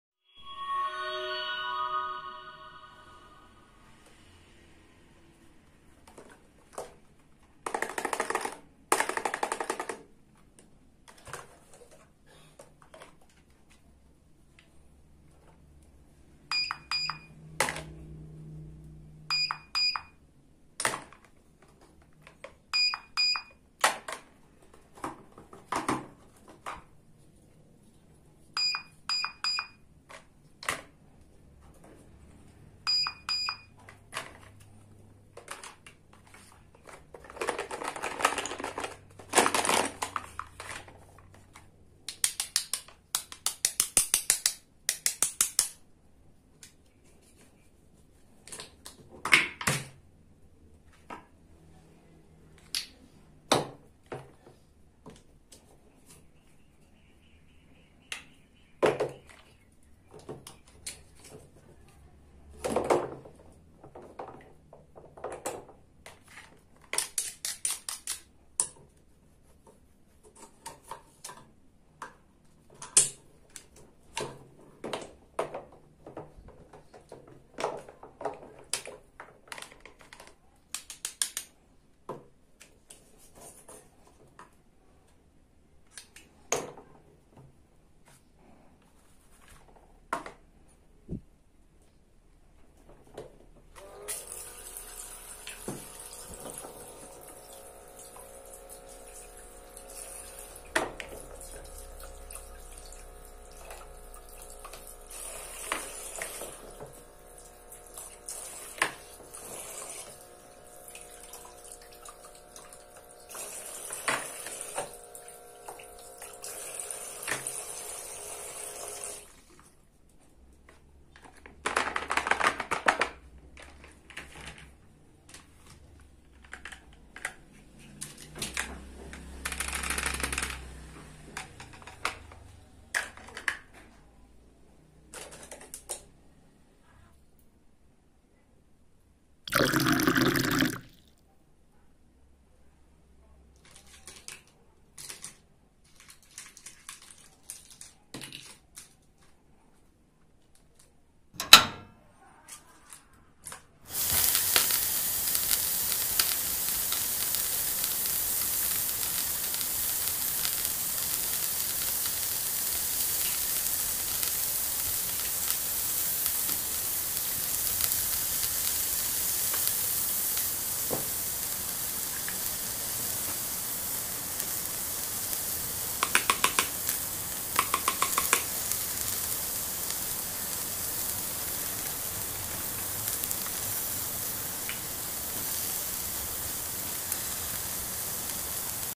Satisfying Video | How To Cutting Vegetables And Fruit | Unboxing Hello Kitty ASMR 27
relax and unwind with this ASMR fresh cut fruits & vegetables experience. Watch and listen as juicy fruits and crisp vegetables are sliced and diced, creating a satisfying and calming sensation. Let the soothing sounds take you on a journey to tranquility. Perfect for those looking for a relaxing ASMR experience.